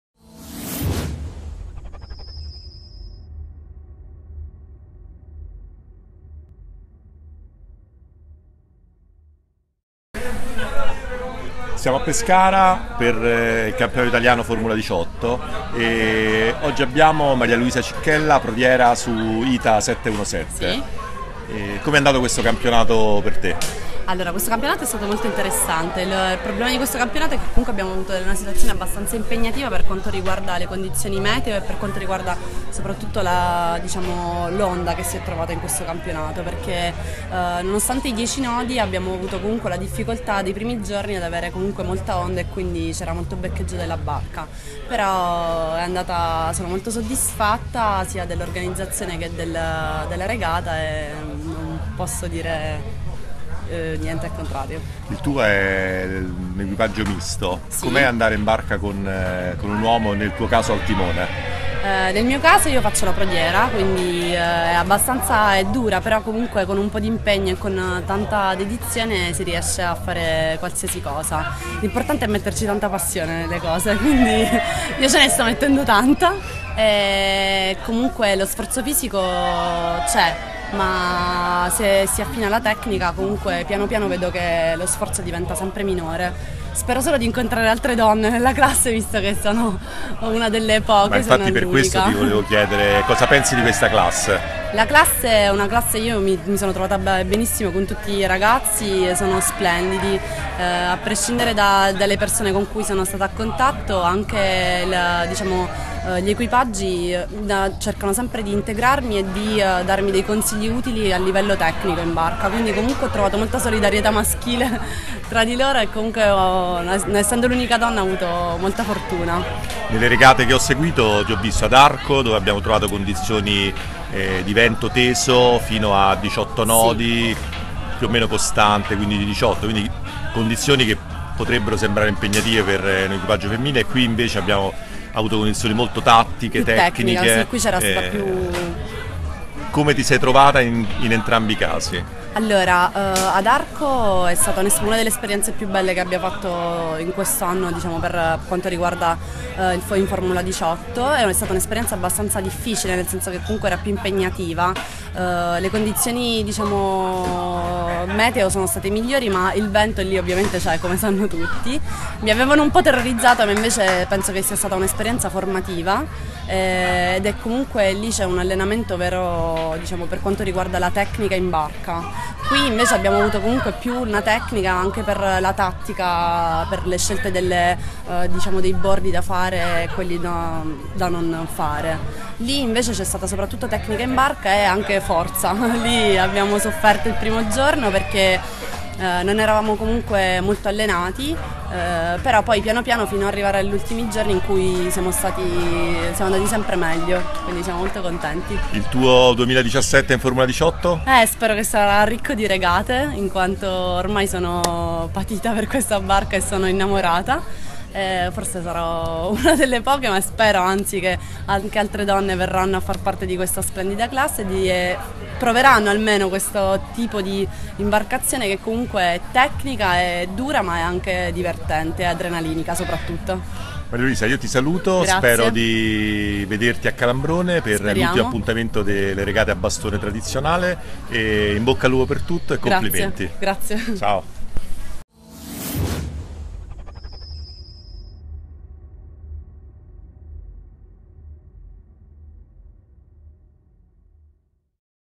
Maria Luisa Cicchella prodiere su ITA 717 al Campionato Nazionale Formula18
Campionato Nazionale Formula18 - Svagamente Pescara 2016